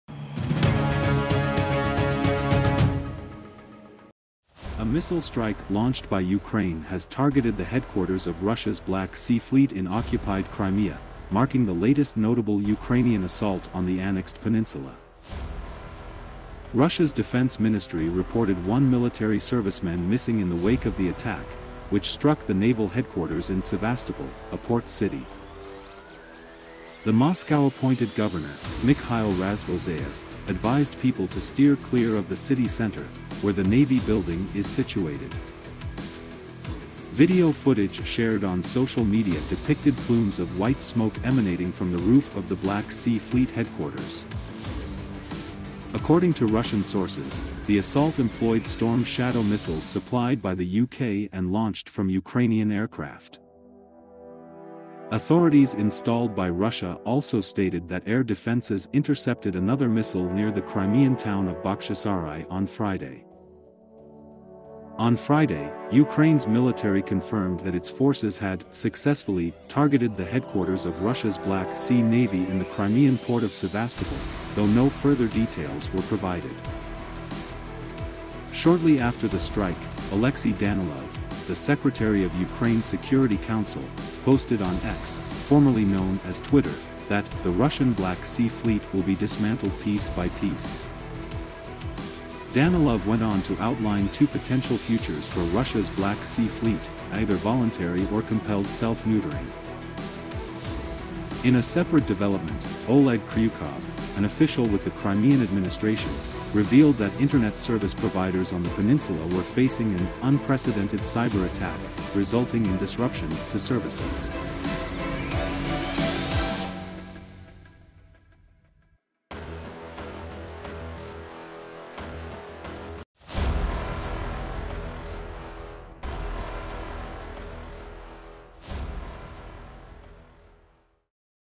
Breaking News: Ukraine destroys Russian Black Sea Fleet HQ in Sevastopol, Crimea
A missile strike launched by Ukraine has targeted the headquarters of Russia's Black Sea fleet in occupied Crimea, marking the latest notable Ukrainian assault on the annexed peninsula. Russia's defense ministry reported one military serviceman missing in the wake of the attack, which struck the naval headquarters in Sevastopol, a port city.

The Moscow-appointed governor, Mikhail Razvozhayev, advised people to steer clear of the city center, where the navy building is situated. Video footage shared on social media depicted plumes of white smoke emanating from the roof of the Black Sea fleet headquarters. According to Russian sources, the assault employed Storm Shadow missiles supplied by the UK and launched from Ukrainian aircraft. Authorities installed by Russia also stated that air defenses intercepted another missile near the Crimean town of Bakhchysarai on Friday.

On Friday, Ukraine's military confirmed that its forces had "successfully" targeted the headquarters of Russia's Black Sea navy in the Crimean port of Sevastopol, though no further details were provided. Shortly after the strike, Oleksiy Danilov, the secretary of Ukraine's security council, posted on X (formerly known as Twitter) that "the Russian Black Sea fleet will be dismantled piece by piece."

Danilov went on to outline two potential futures for Russia's Black Sea fleet: either voluntary or compelled self-neutering. In a separate development, Oleg Kryuchkov, an official with the Crimean administration, revealed that internet service providers on the peninsula were facing an "unprecedented cyberattack," resulting in disruptions to services.